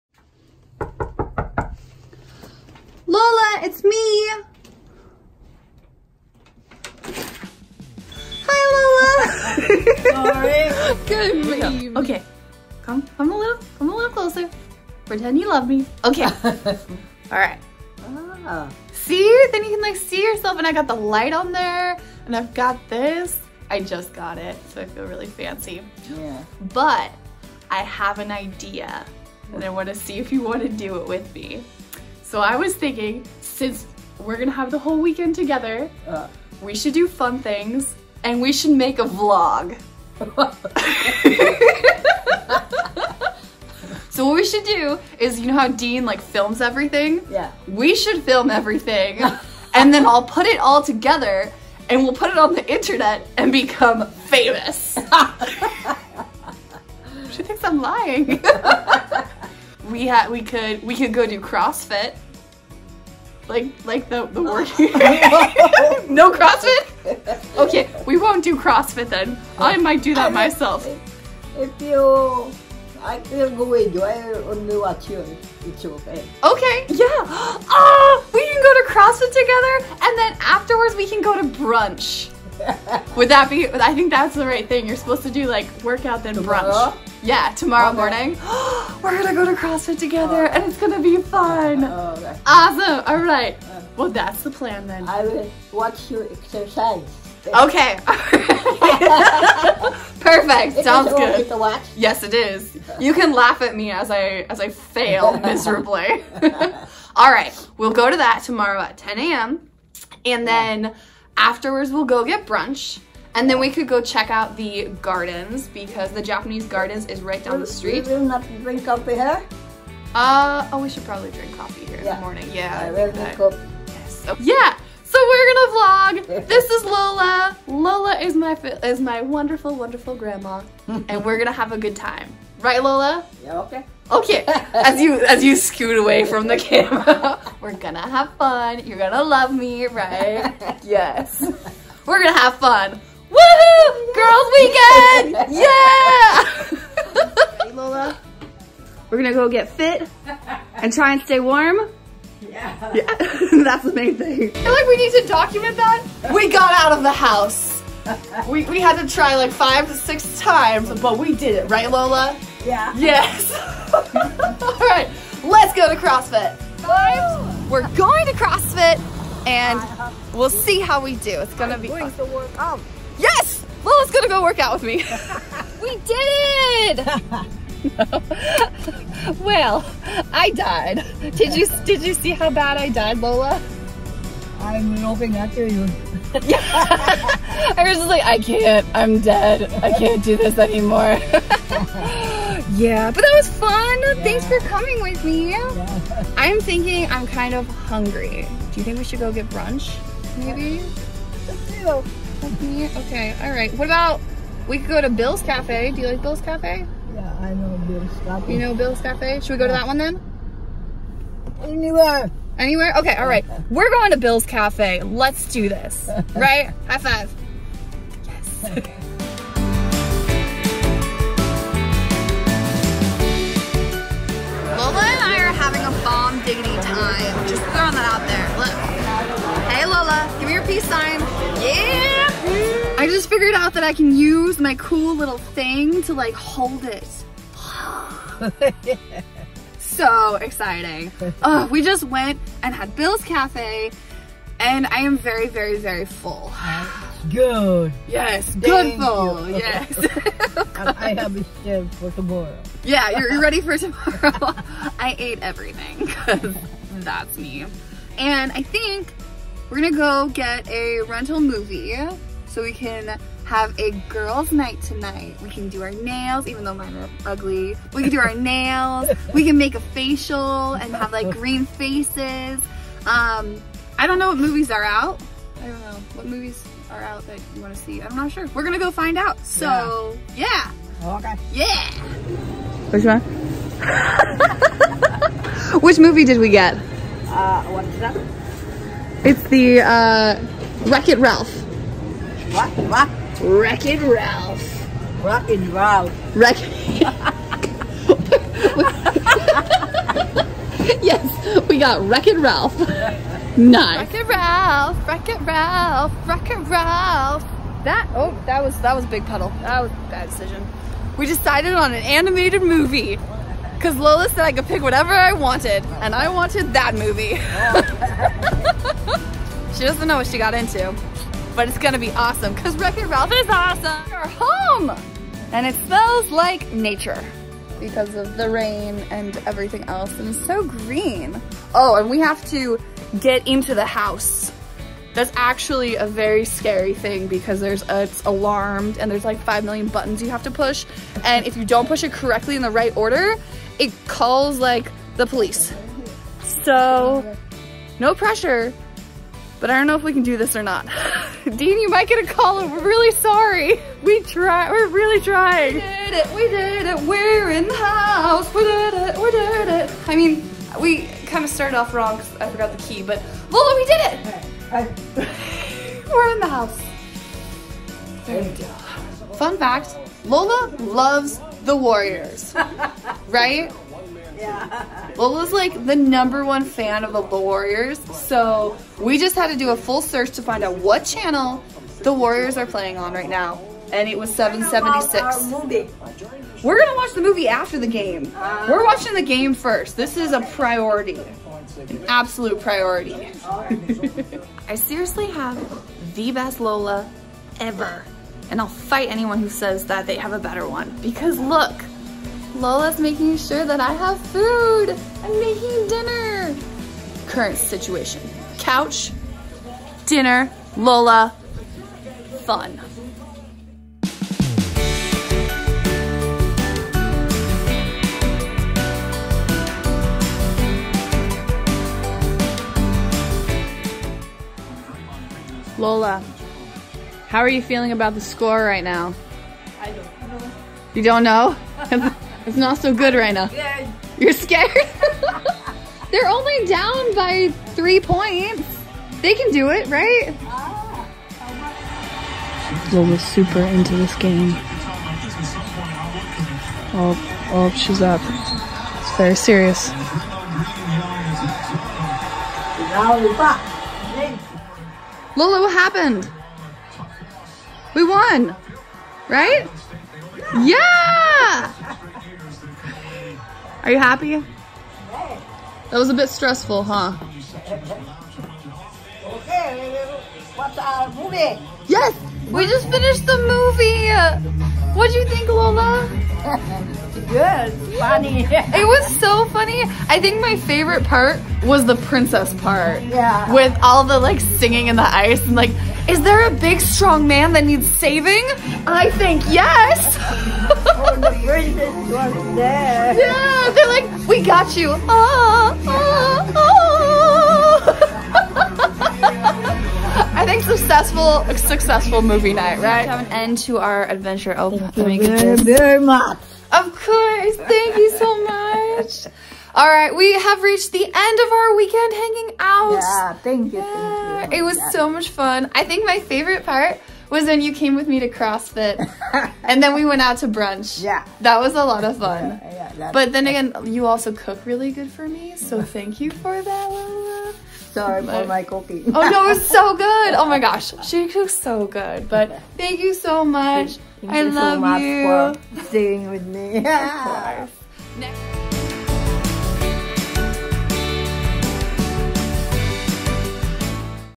Weekend With Lola!
Want to know what I do on a dreary, rainy, weekend? Hang out with my Lola, of course! Lola is a Tagalog work meaning “grandmother”! Technically, I think she’s my great aunt, but Lola is way easier, and fun, than great aunt!

Enjoy our silly adventures and let us know what you’d like for us to do next!

Where you can find out more about me:
Check our my Instagram: @tabithathetourist